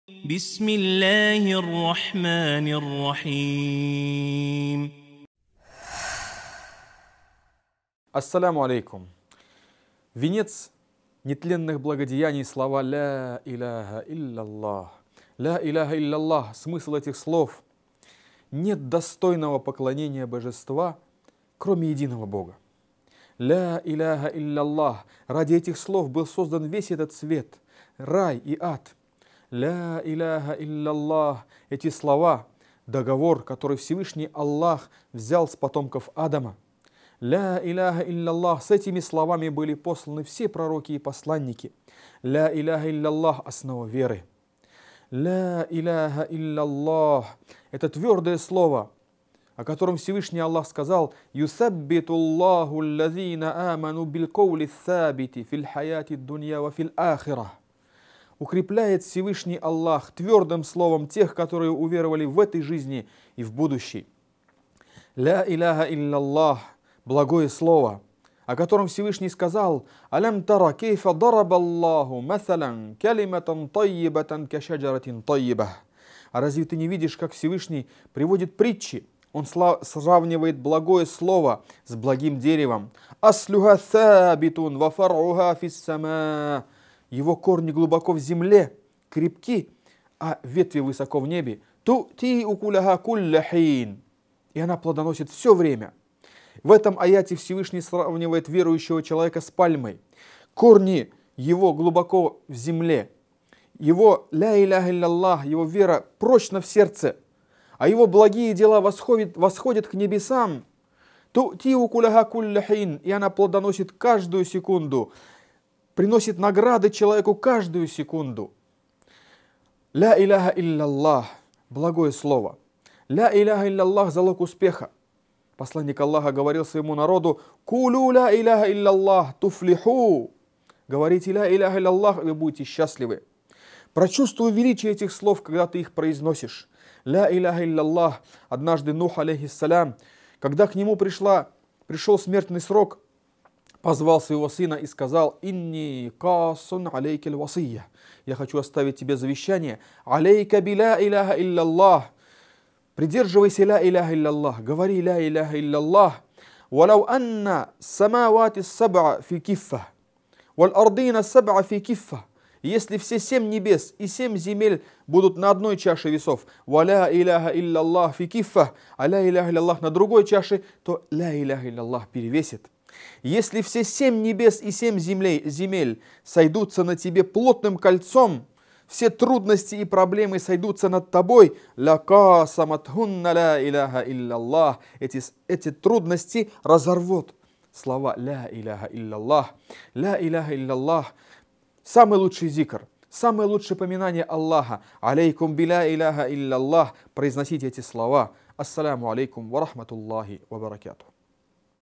Ля иляха илля Ллах
Ля иляха илля Ллах – венец нетленных благодеяний! Великие и важнейшие слова в жизни мусульманина. Повторяй их, осознавая смысл, и не будешь опечален. Повторяй и будешь избавлен от бед!